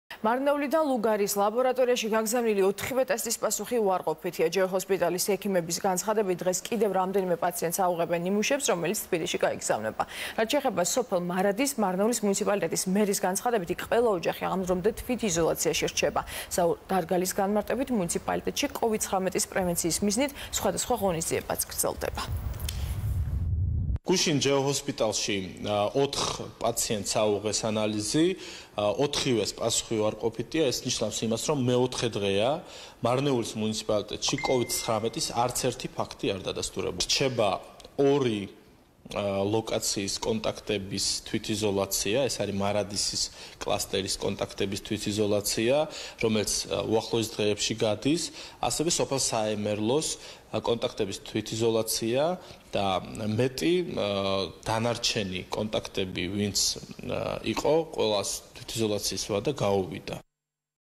TV - საზოგადოებრივი მაუწყებელი - ვითარება მარნეულის მუნიციპალიტეტში - 21.04.2020 - 15:12
მარნეულიდან ლუგარის ლაბორატორიაში გაგზავნილი ოთხივე ტესტის პასუხი უარყოფითია. ჯეოჰოსპიტალის ექიმების განცხადებით დღეს კიდევ რამოდენიმე პაციენტს აუღებენ ნიმუშებს რომელიც თბილისში გაიგზავნება. რაც სოფელ მარადისში ყველა ოჯახი, ამ დრომდე, თვითიზოლაციაში რჩება. მუნიციპალიტეტის მასშტაბით „კოვიდ-19“-ის პრევენციის მიზნით სხვადასხვა ღონისძიებები ხორციელდება.